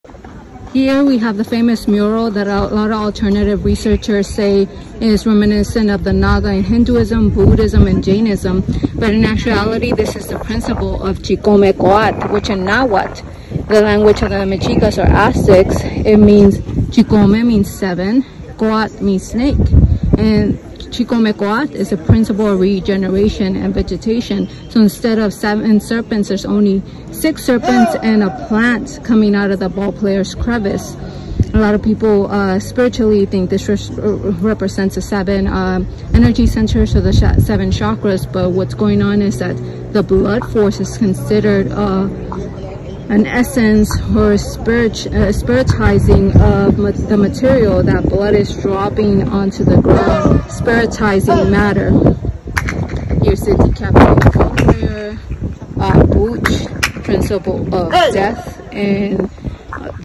Ancient Photoshop @Chichen Itza’s Ball Court 🏐 Mural
The number seven keeps showing up a lot at Chichen Itza’s massive ball court.

As if  playing a game of hide-and-seek. Waiting to be discovered by the avid seeker.

Seven is seen in the well known ball court depiction of the beheaded ball player who represents Chicome-Coatl.

Chicome-Coatl is the Mexica or Aztec energy of agriculture and rebirth.

&  there is also;

Seven ball players who are held captive here on these walls
Seven is also the number of the snake
Seven is the number of times that an echo flutter is heard when you clap once.

The most important connection is that seven is the union of spirit in matter.

When spirit dives into matter, it manifests spiritual things into our tangible 3D world.

This sacred connection s fitting of one of the 7 wonders of the world.

Notes:

Western alt-researchers suggest that the origin of Chicome Coatl’s creation is linked to other cultures and they label this concept “diffusionism.”

However, attributing these ideas to traveling “culture seeders”  who came to Turtle Island to “ civilize” the indigenous people of this continent, is problematic. It wrongly implies that indigenous people of the “Americas” didn’t independently develop their own unique contributions.

I disagree with the notion that Chicome Coatl is associated with the Indian Naga concept. While both cultures have beautiful traditions, there is no historical connection between them.

Mesoamerican traditions had their own energy center systems.

Although this is a Maya site, it has a heavy Toltec influence from which the Aztecs heavily borrowed ideas from.